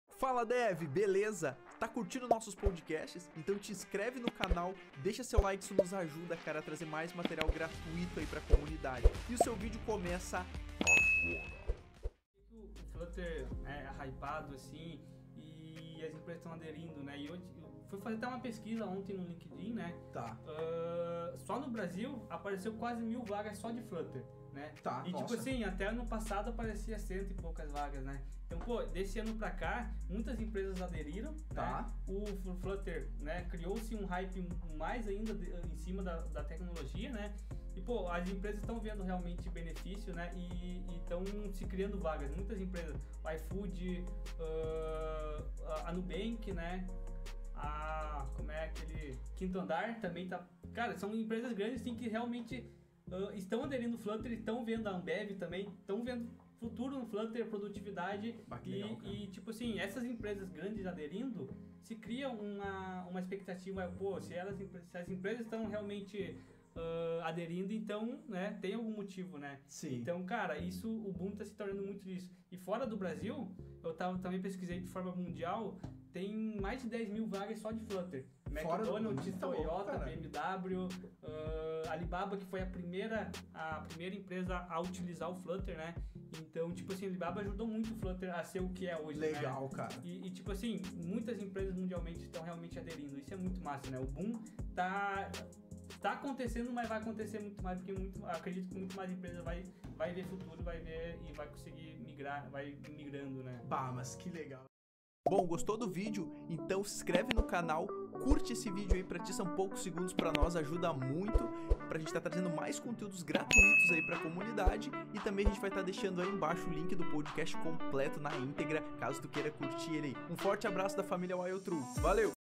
4 EMPRESAS QUE NÃO RESISTIRAM AO FLUTTER | Cortes Whilecast
Neste Corte de Podcast falamos quais empresas usam Flutter.